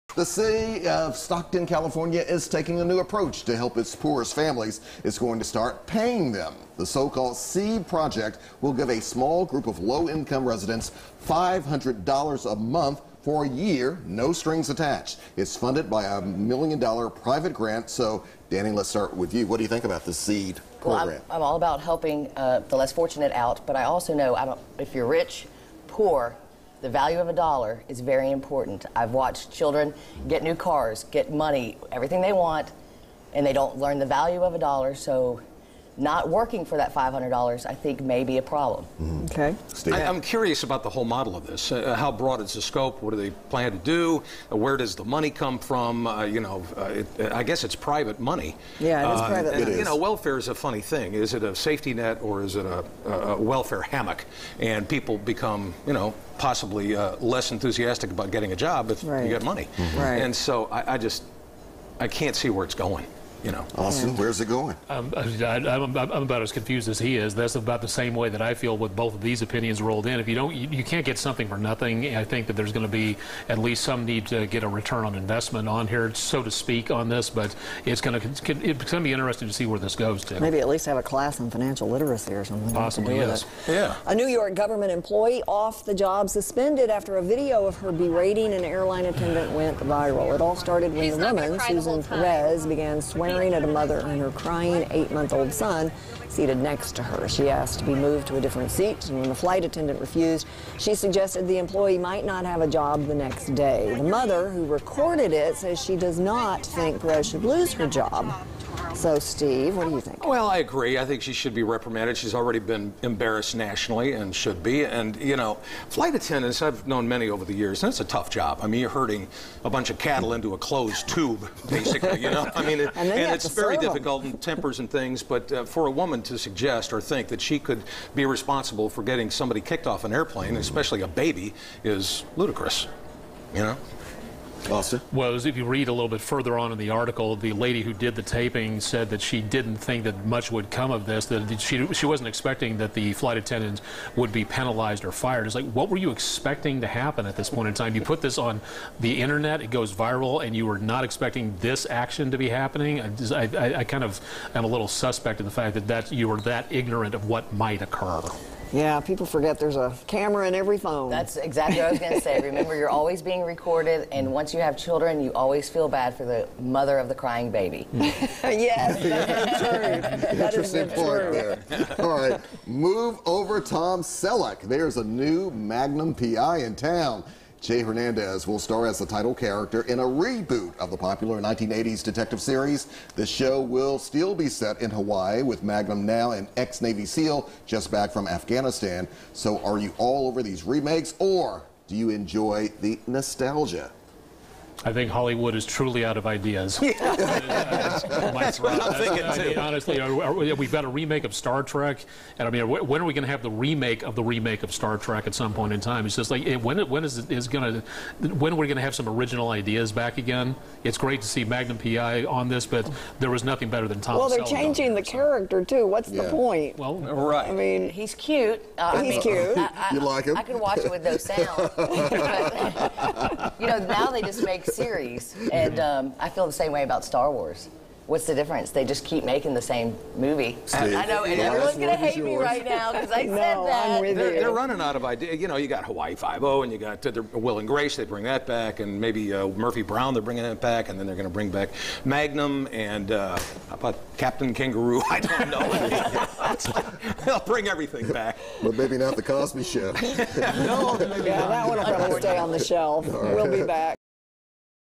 Watercooler Wednesday
Danni Bruns, Steve Conley and Austen Onek talk about the controversial idea to pay poor families, the viral airplane video and the Magnum PI reboot.